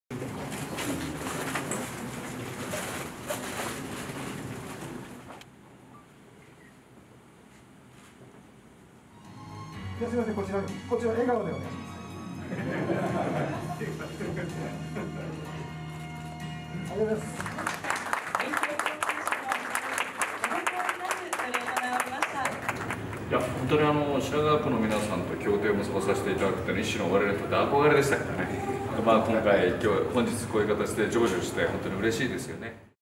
龍馬が縁で協定締結＝高知県と東京都品川区
幕末の志士、坂本龍馬にゆかりがある高知県と東京都品川区は７日、文化と観光、産業などの分野で相互交流することなどを盛り込んだ連携協定を締結した。【時事通信映像センター】